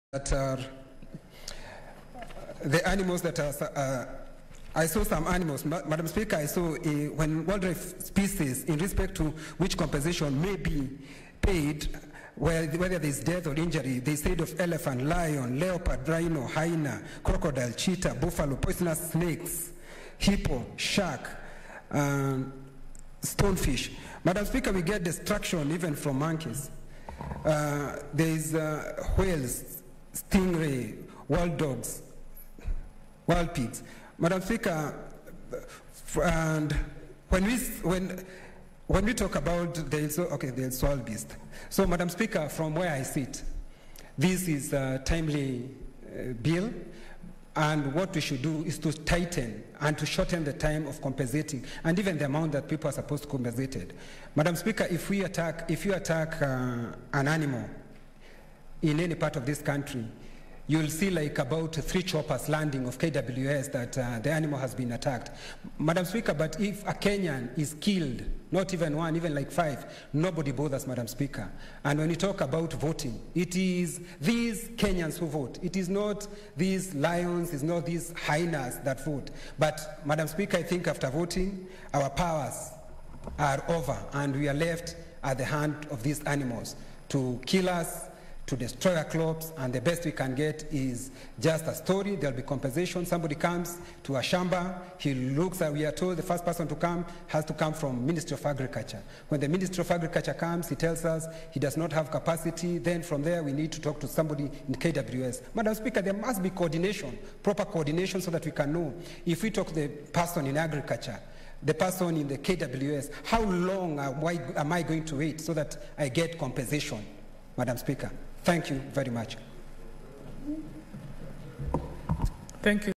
Senator Nderitu says we need to tighten the time of compensating victims of human wildlife conflict
Senate Proceedings